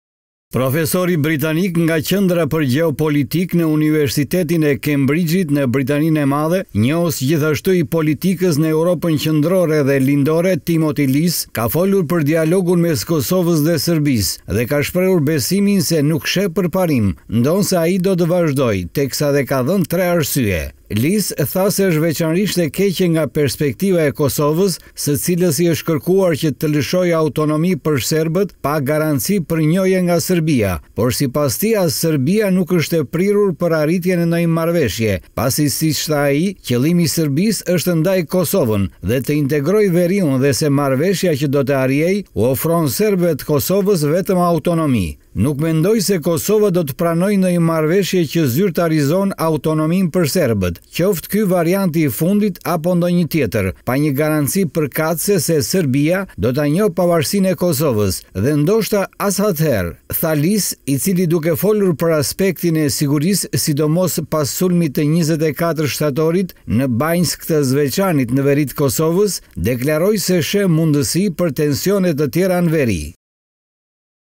Kosova s’do të pranojë marrëveshje me Serbinë/ Flet profesori britanik
Profesori britanik nga Qendra për Gjeopolitikë në Universitetin Cambridge në Britani të Madhe, njohës i politikës së Evropës Qendrore dhe Lindore, Timothy Less ka folur për dialogun mes Kosovës dhe Serbisë dhe ka shprehur besimin se nuk sheh përparim, ndonëse ai do të vazhdojë, teksa edhe ka dhënë tre arsye.

𝐀𝐮𝐭𝐨𝐫 : NTV